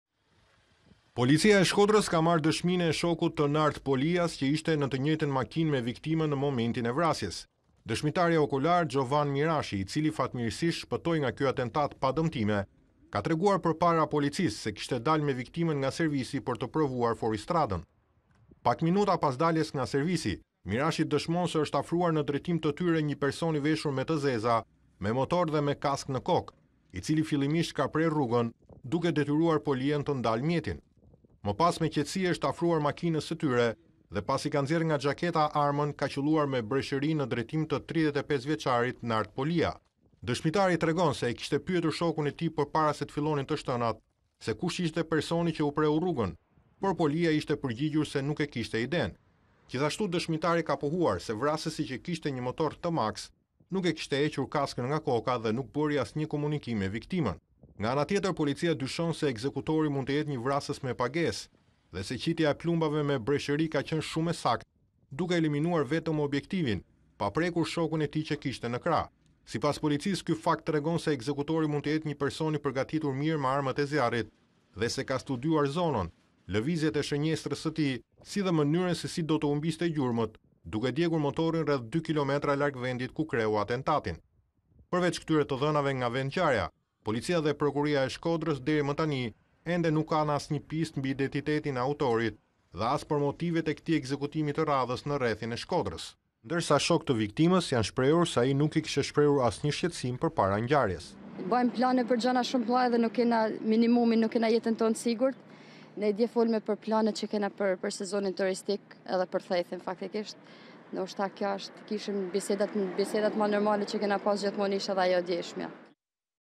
Vrasja në Shkodër, shoku i viktimës tregon dinamikën e krimit
Policia e Shkodrës ka marrë dëshminë e shokut të Nard Polias që ishte në të njëjtën makinë me viktimën në momentin e vrasjes.
Dëshmitari okular Xhovan Mirashi, i cili fatmirësisht shpëtoi nga ky atentat pa dëmtime, ka treguar përpara policisë se  kishte dalë me viktimën nga servisi për të provuar fuoristradën. Pak minuta pas daljes nga servisi, Mirashi dëshmon se është afruar në drejtim të tyre një person i veshur me te zeza me motor dhe me kaskë në kokë, i cili fillimisht ka prerë rrugë duke detyruar Polian të ndalë mjetin.
Më pas me qetësi i është afruar makinës së tyre dhe pasi ka nxjerre nga xhaketa armën ka qëlluar me breshëri në drejtim të 35 vjeçarit Nard Polia. Dëshmitari tregon se e kishte pyetur shokun e tij se kush ishte personi që u preu rrugën, por Polia i ishte përgjigjur se nuk e kishte idenë. Gjithashtu dëshmitari ka pohuar se vrasësi që kishte një motor T Max, nuk e kishte hequr kaskën nga koka dhe nuk beri asnjë komunikim me viktimën.
Nga ana tjetër policia dyshon se ekzekutori mund të jetë një vrasës me pagese dhe se qitja e plumbave me breshëri ka qenë shumë e saktë, duke eleminuar vetëm objektivin pa prekur shokun e tij që kishte në krah.
Sipas policisë ky fakt tregon se ekzekutori mund të jetë një person i përgatitur mirë me armët e zjarrit dhe se ka studiuar zonën, lëvizjet e shënjestrës së tij si dhe mënyrën se si do të humbte gjurmët duke djegur motorin rreth 2 km larg vendit ku kreu atentatin.
Përveç këtyre të dhënave nga vendngjarja, policia dhe prokuroria e Shkodrës ende nuk kanë asnjë pistë mbi identitetin e autorit dhe as për motivet e këtij ekzekutimi të radhës në rrethin e Shkodrës.